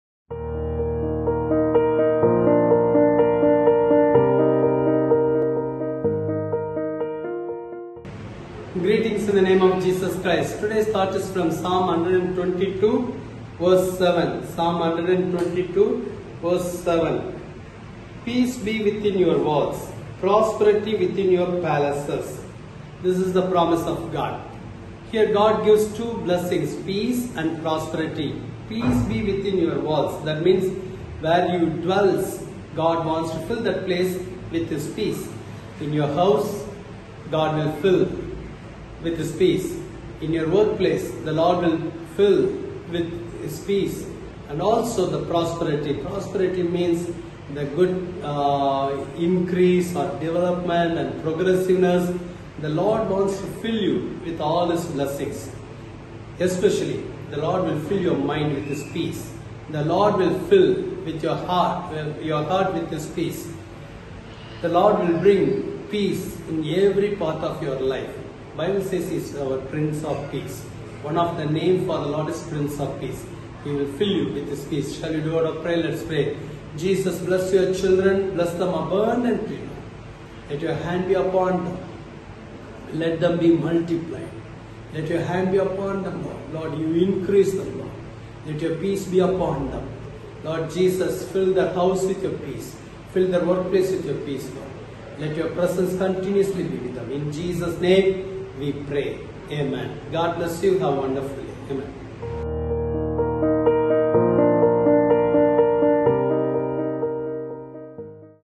Peace be within your walls (Psalms‬ ‭122:7‬) - Thought for the Day - 11Apr2022
“Peace be within your walls, Prosperity within your palaces.” (Psalms‬ ‭122:7‬)

Thought for the Day - 11Apr2022